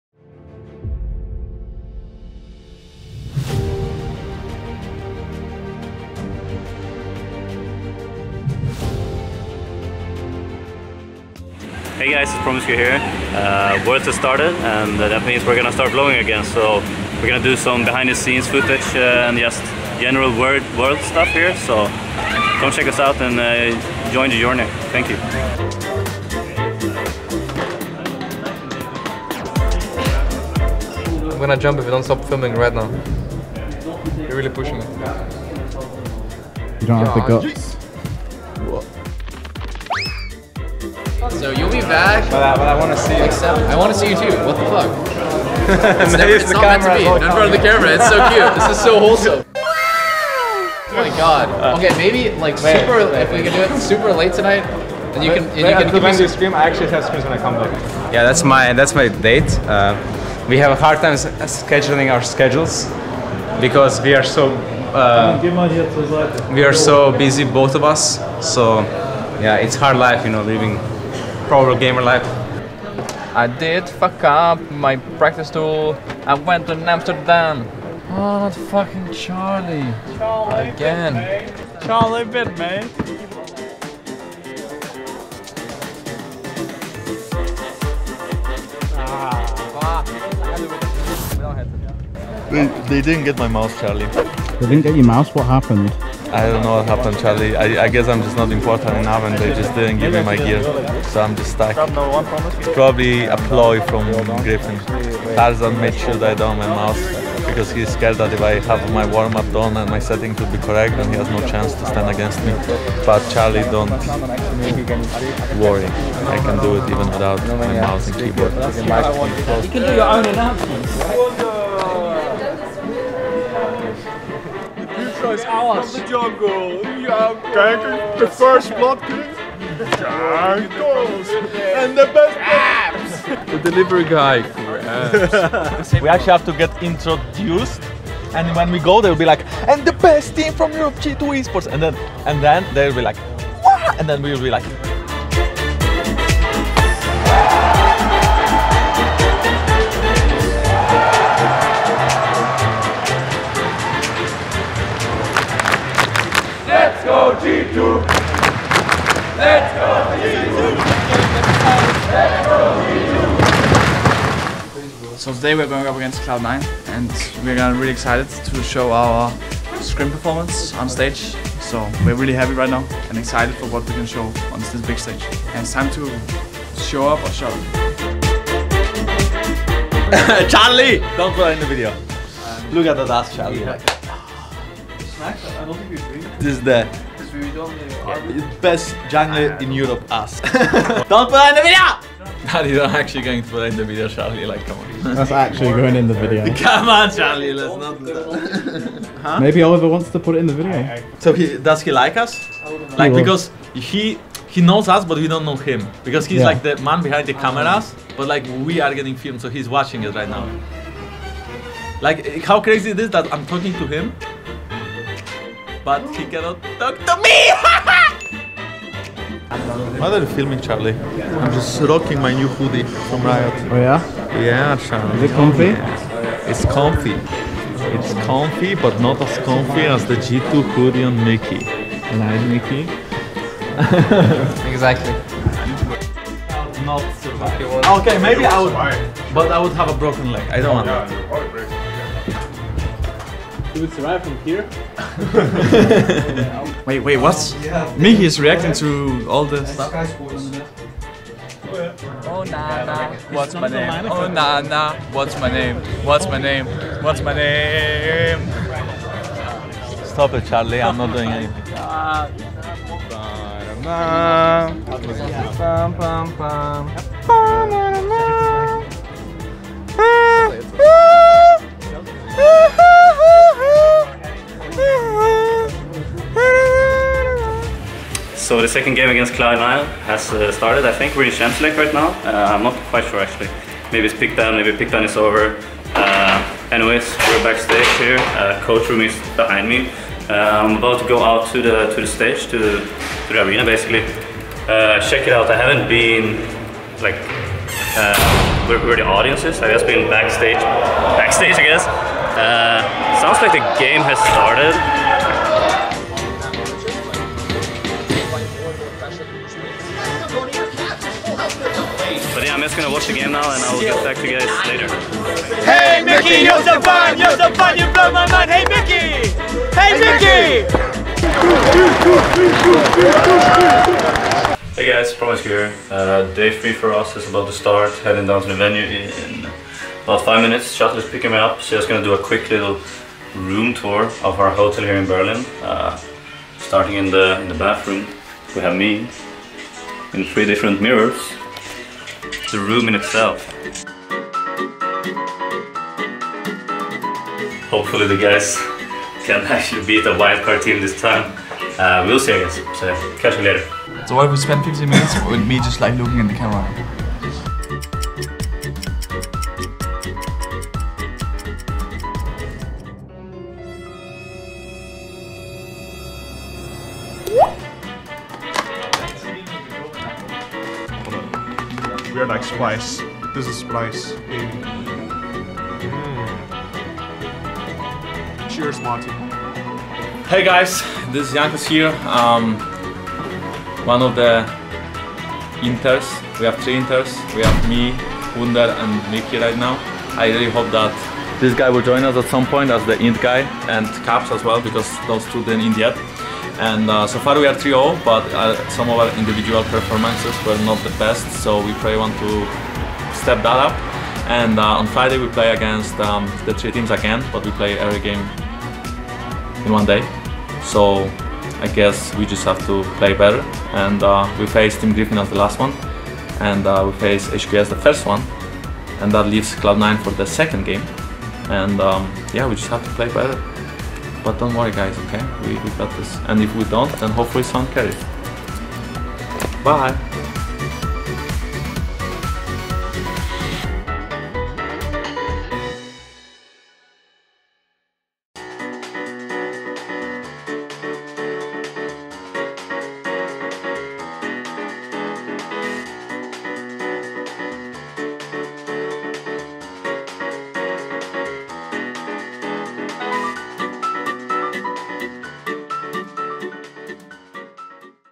Show Up or Shut Up! | G2 at Worlds 2019
It's time for us to show up or shut up! Want to see what the boys have been getting up to behind the scenes at Worlds2019? Well now you can, powered by Logitech G!